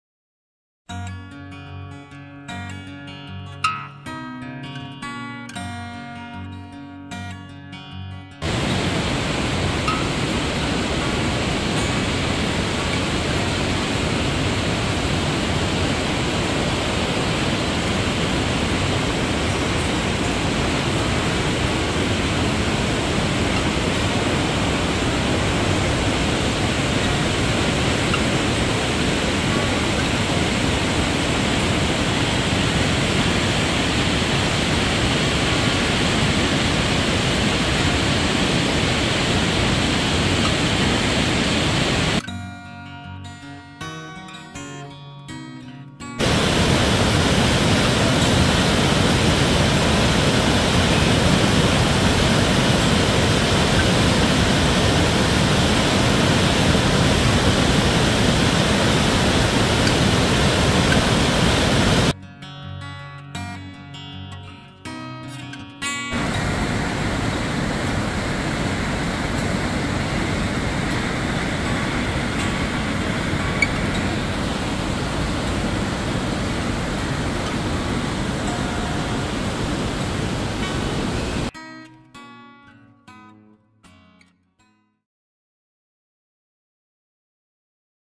The Last Spike & Crazy Creek Suspension Bridge
Took a rip down the highway to see what I could see. Stopped in at The Last Spike and Crazy Creek Suspension Bridge.